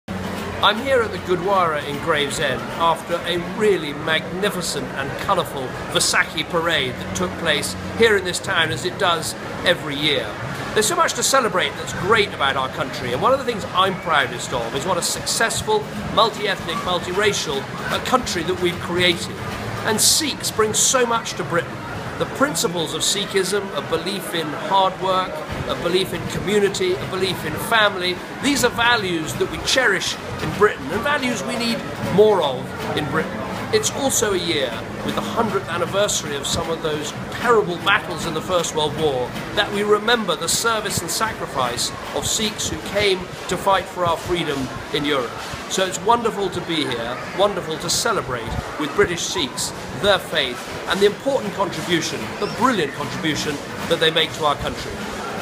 David Cameron: My message from the Vaisakhi parade
My video from the Guru Nanak Darbar Gurdwara in Gravesend - after a really magnificent, colourful Vaisakhi parade: